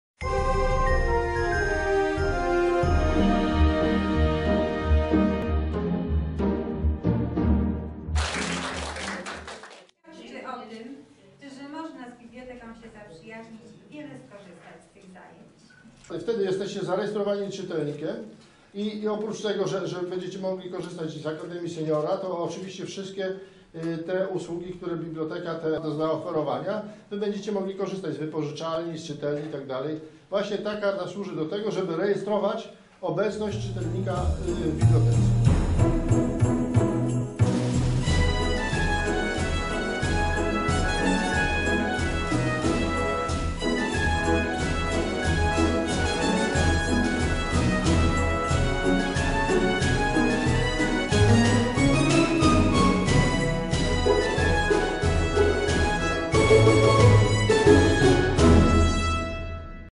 Akademia Seniora otwarcie nowej edycji kursów 2014/2015
W Miejskiej Bibliotece Publicznej w Galerii Format odbyło się spotkanie organizacyjne dla nowych kursantów Akademii Seniora. Była to okazja nie tylko do zaprezentowania tematyki i organizacji kursów, ale też do zapoznania się uczestników.